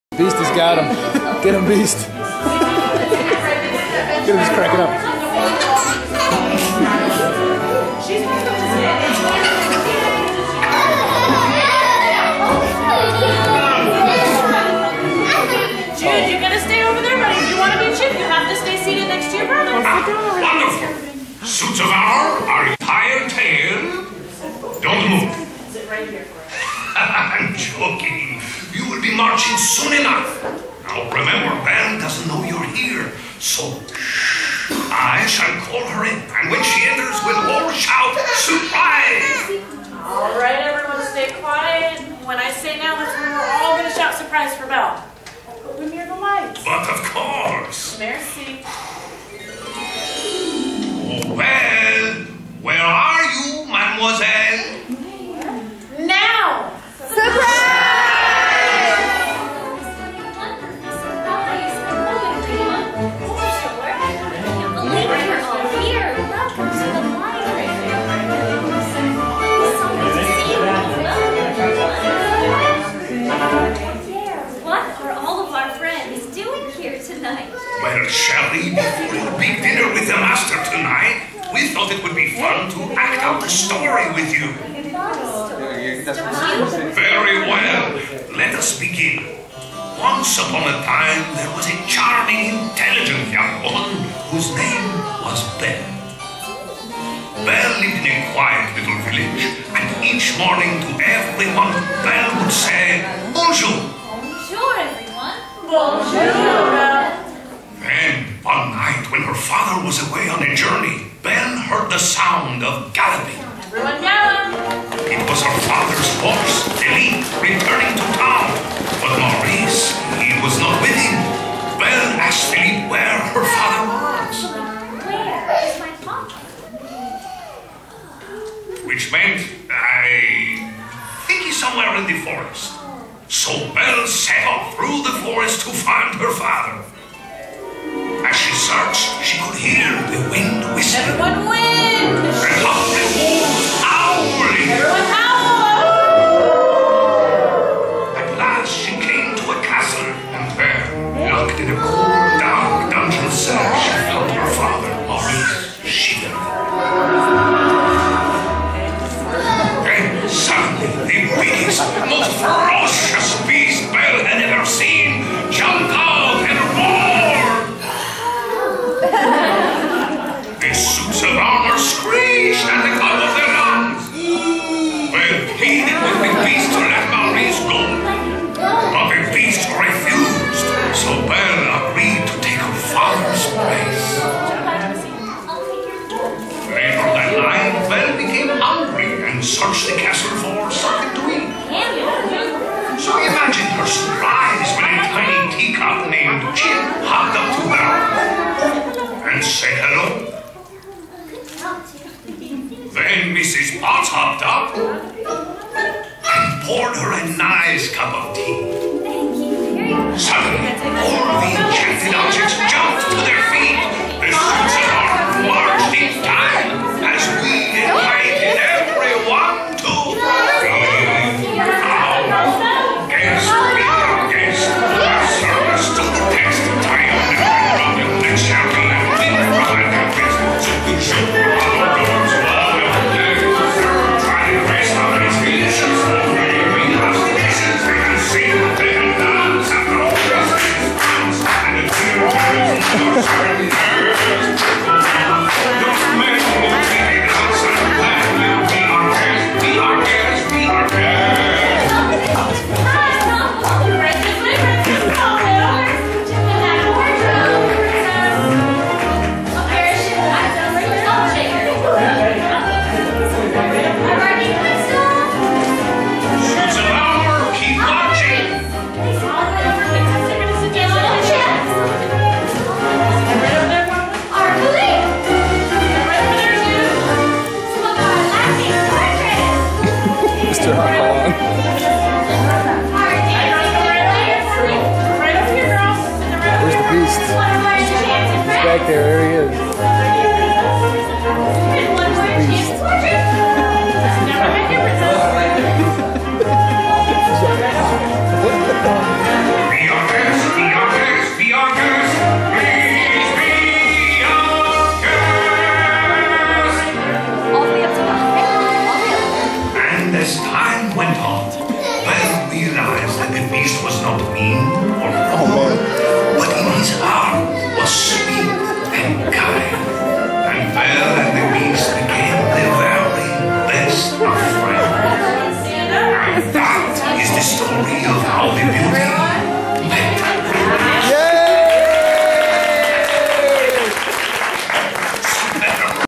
beauty and the beast1
Theatrical debut of our children.  I love it when the Disney cast member threatens Jude that she won't let him be Chip if he keeps acting silly.  I think the parents would have revolted if she would have tried to take Chip away from the Make a Wish kid! :)